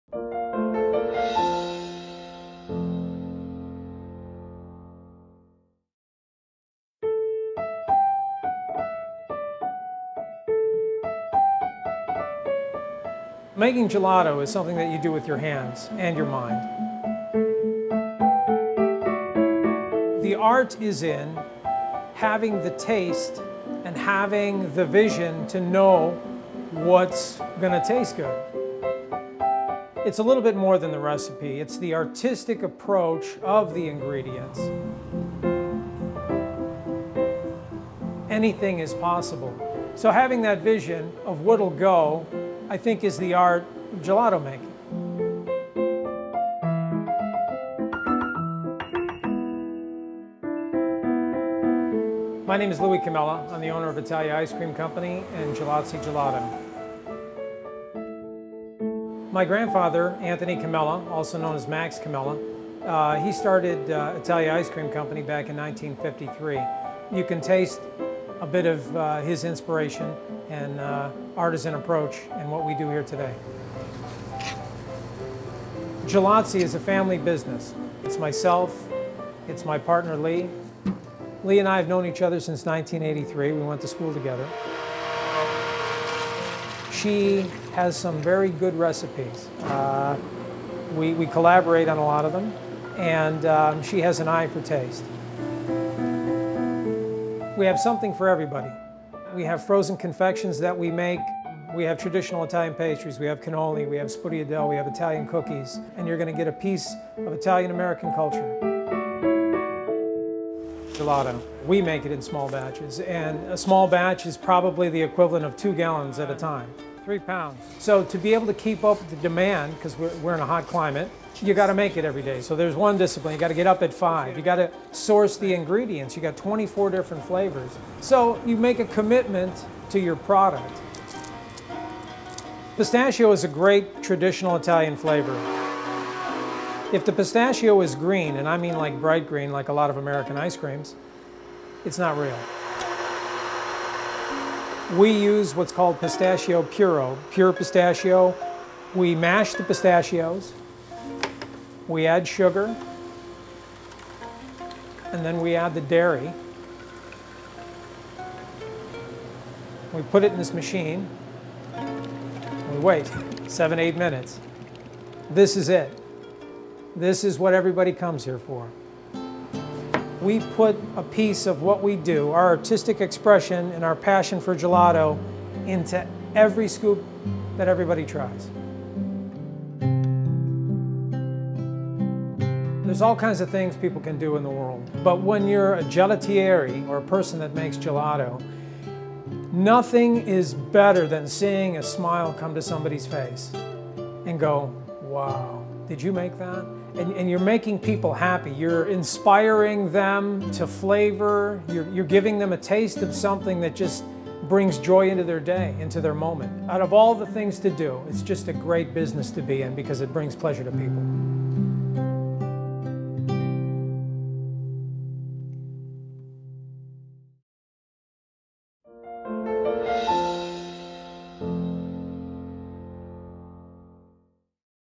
Gelazzi Gelato
Louie Comella, owner of Gelazzi (a popular gelato shop in the Houston Heights), takes us through the artful process of making delicious, quality gelato.